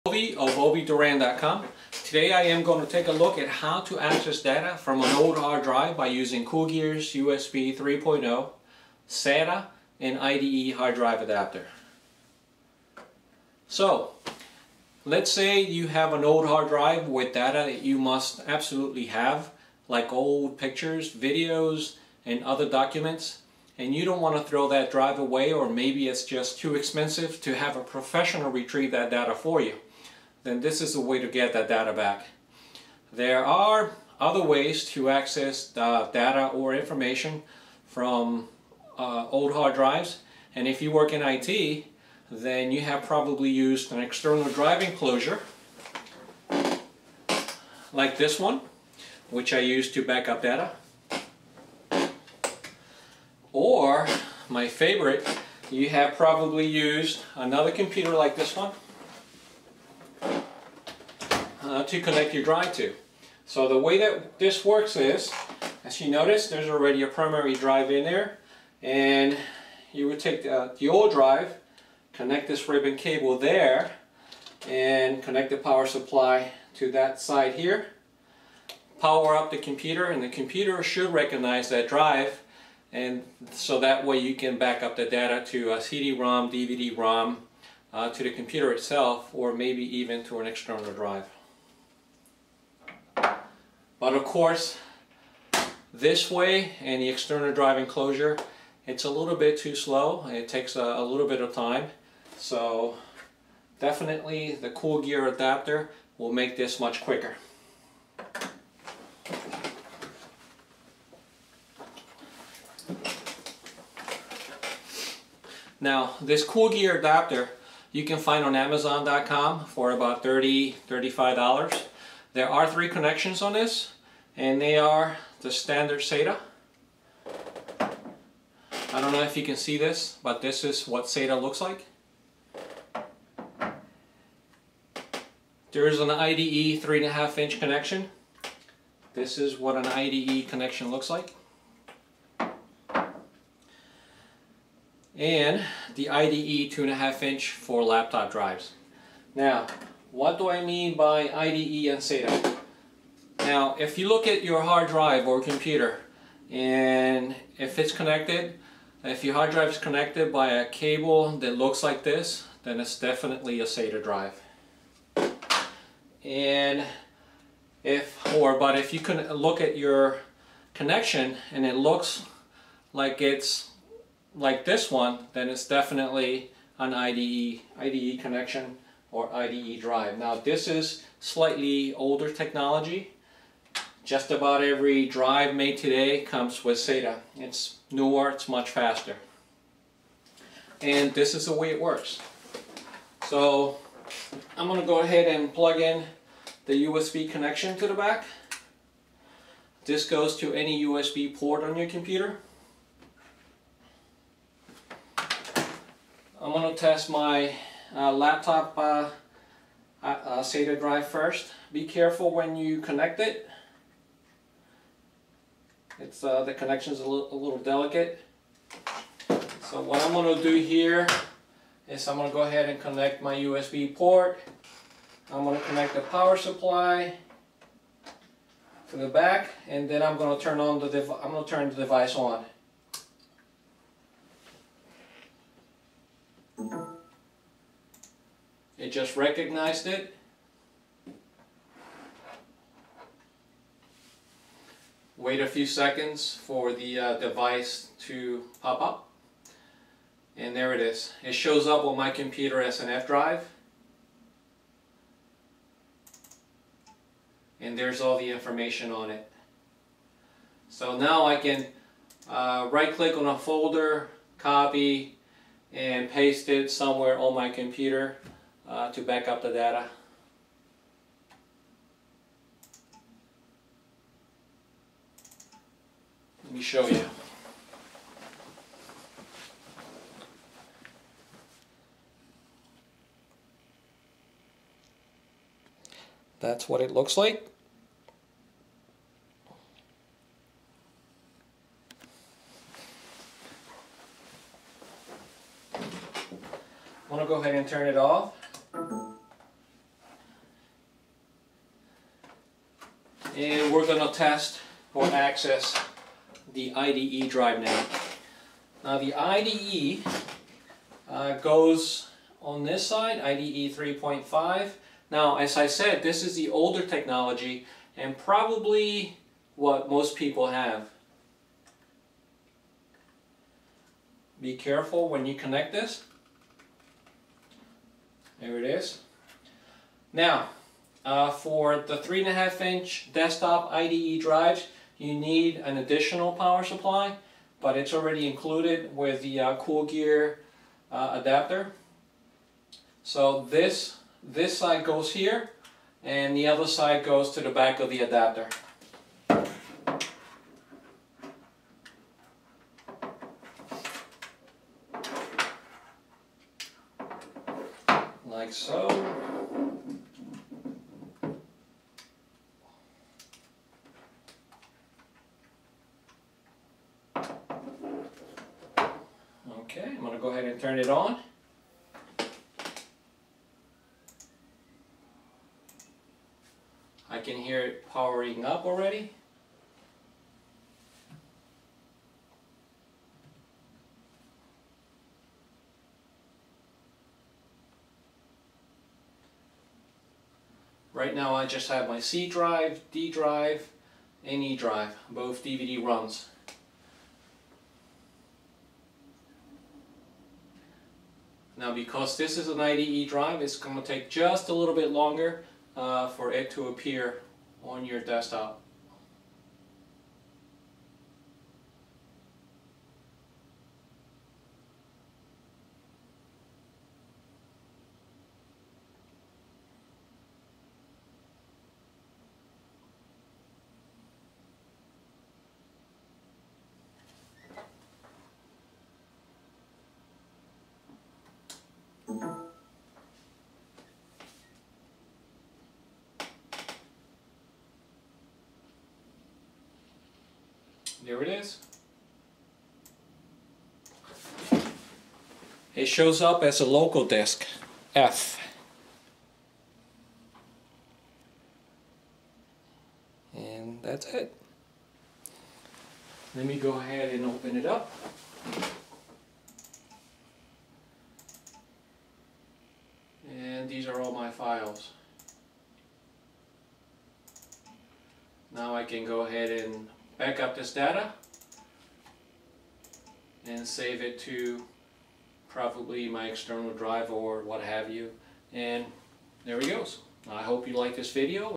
USB Hard Drive Adapter for SATA and IDE
How to recover data from an old hard drive using Coolgear's USB 3.0 Hard Drive Adapter.